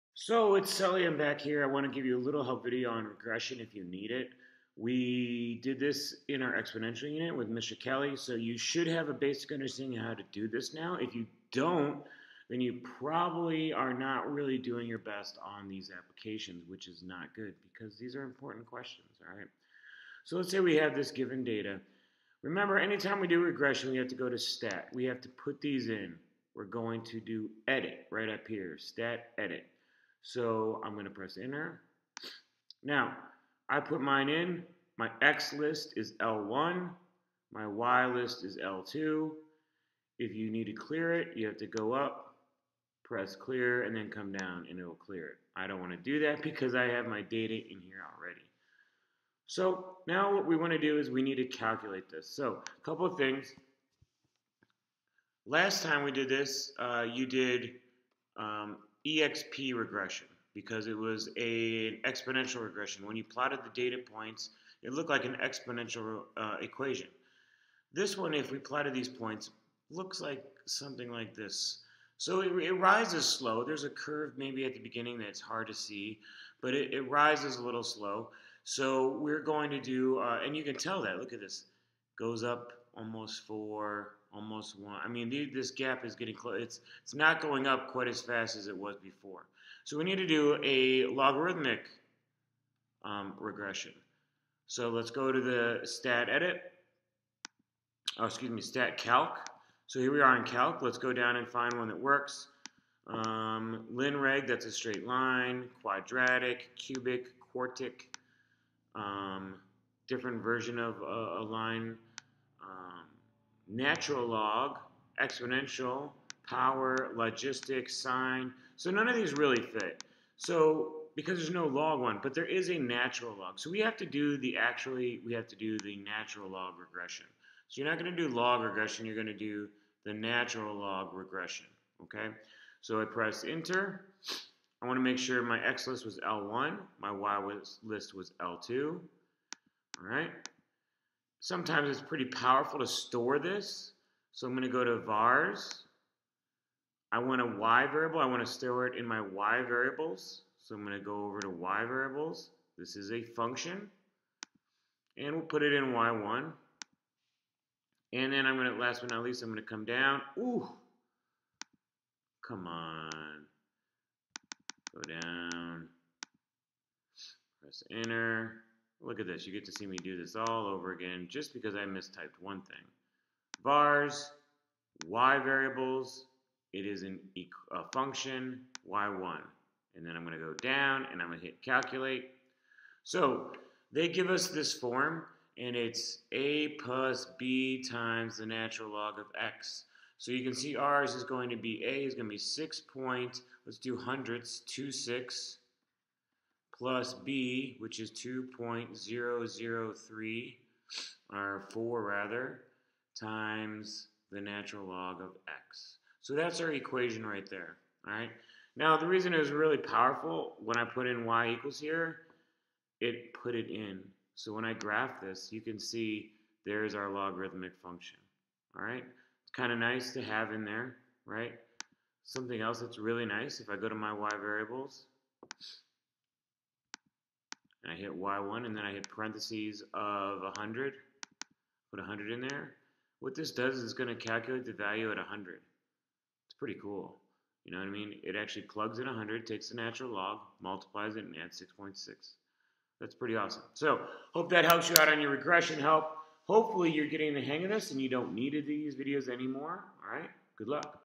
Log Regression Help Video
Use to help you with setting up to do a logarithmic regression for 8.2.